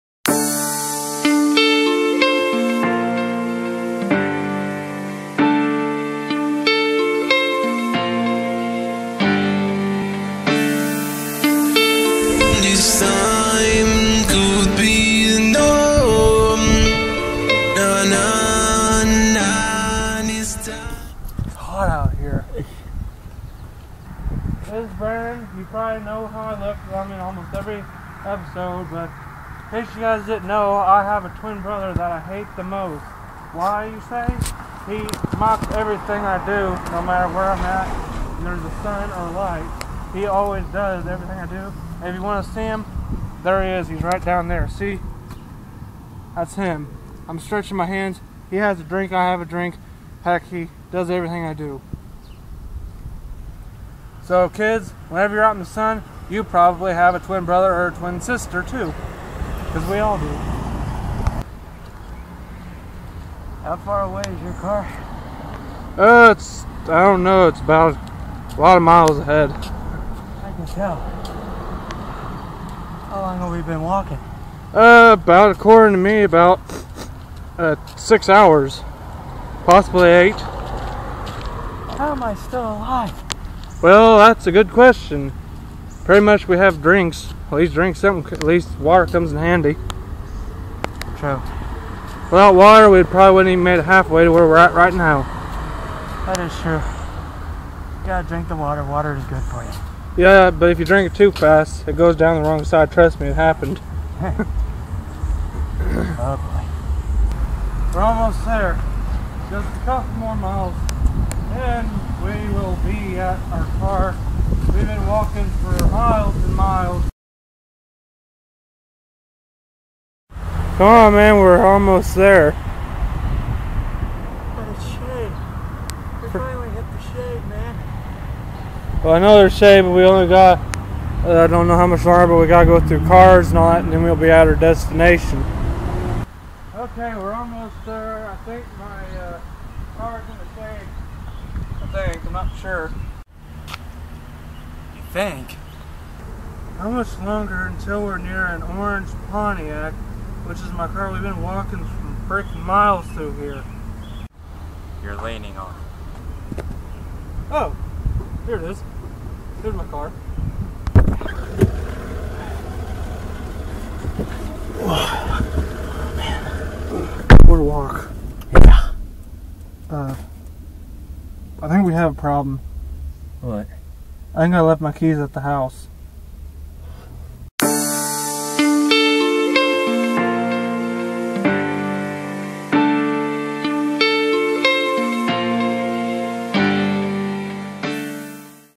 Crazy Vlogs season 3 episode 9: Journey to the car
Brandon and Jamie must get to the car while in the heat of the day